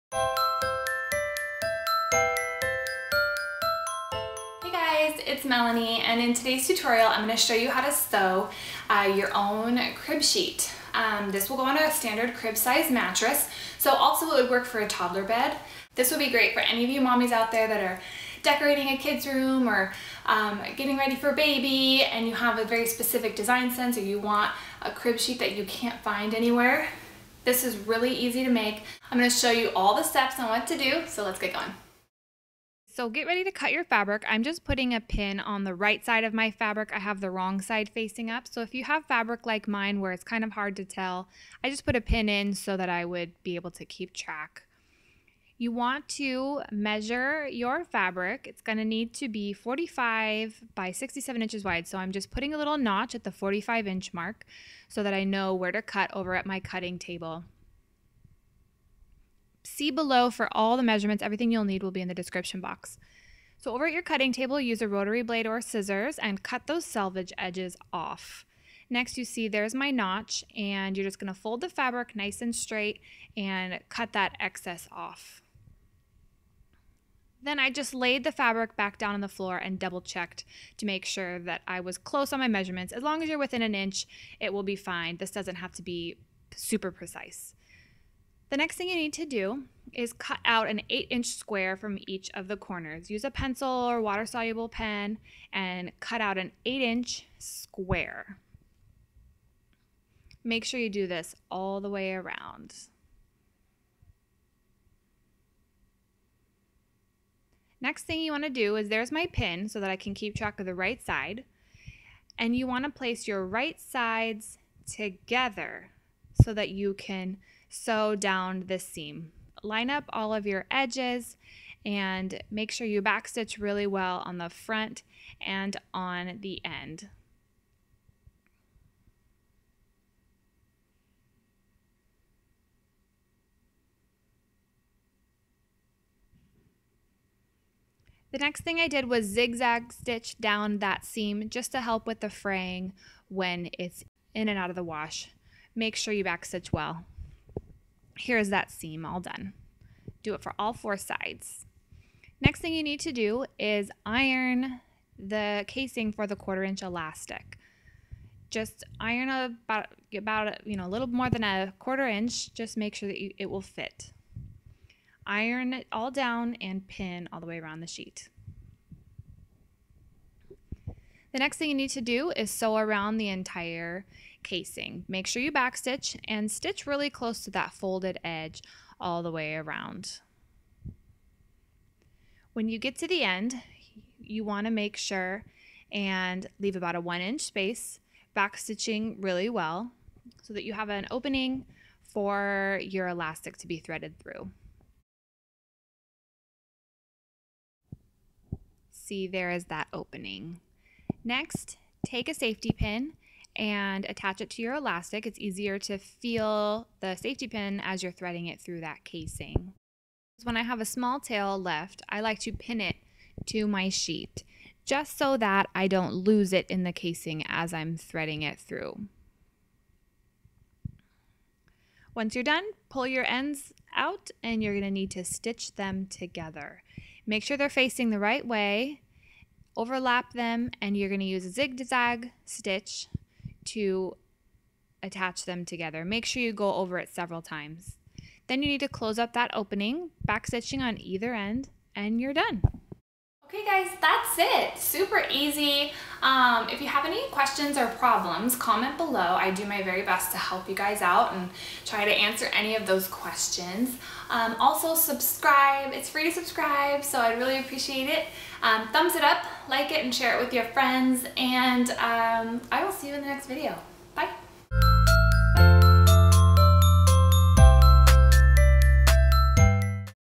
How to Make a Crib Sheet
This video shows you step by step how to make and sew and crib sheet for a standard crib mattress or toddler bed.  This is a great diy project for your baby nursery!  My daughter loves pink and she needed more sheets, so I decided to make her a sheet from a lovely Anna Maria Horner voile fabric.

Materials
2 yards fabric (at least 44 inches wide) 100% Cotton is great!  Mine is Voile. Prewash.
78 inches of 1/4 inch elastic
tape measure
thread
scissors or rotary blade
pins
iron
sewing machine

Standard crib mattress is 52 x 27 x 6
adjust your measurements if your crib is not this size.

Cut your fabric 67 inches by 44/45 inches.
Cut out an 8 inch square in each corner as shown in the video
Right sides together, sew the corners of the sheet and finish the seam with a zig zag
Iron and stitch around the outside of the sheet to create a casing for the elastic
Sew the casing closed to the folded edge leaving a 1 inch space
Using a safety pin, thread the elastic through the casing
Attach the 2 ends of the elastic with a zig zag stitch.
Close the 1 inch gap on the casing